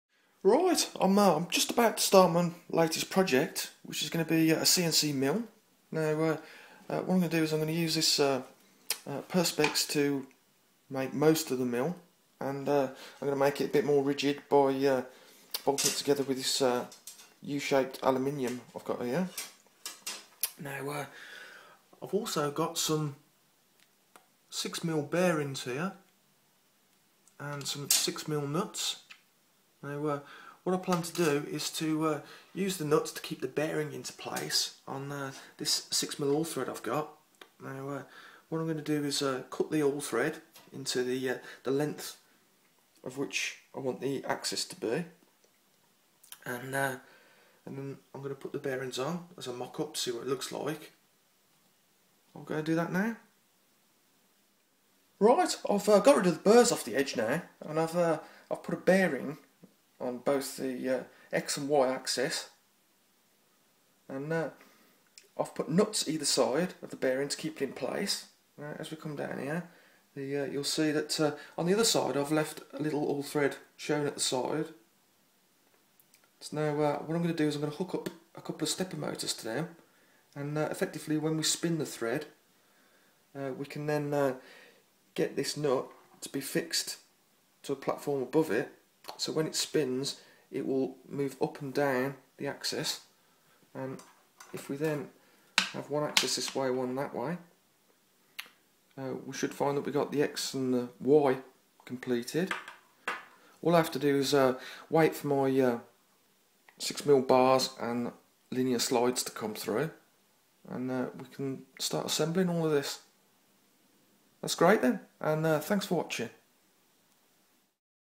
CNC Machine (Start) 01
This is the Start of My Homemade CNC Mill.

I am building this so I can Mill Bits of Plastic and Cut My Own Circuit Boards.

I will be putting a full list of Parts up on my Website.